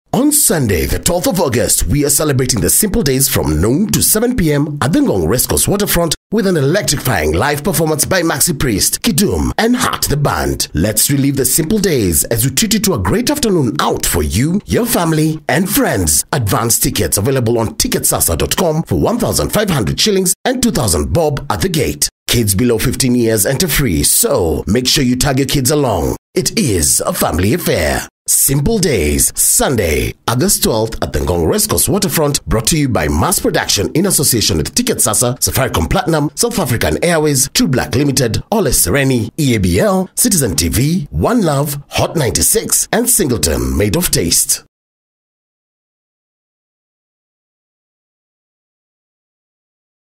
Commercial Voice Actor | African English and Swahili Voice overs
Are you in need of quality African English and Swahili voiceovers? I will be able to provide solutions to your needs.

I deliver in quality WAV or mp3 files, as per your specifications within 24-48 hours.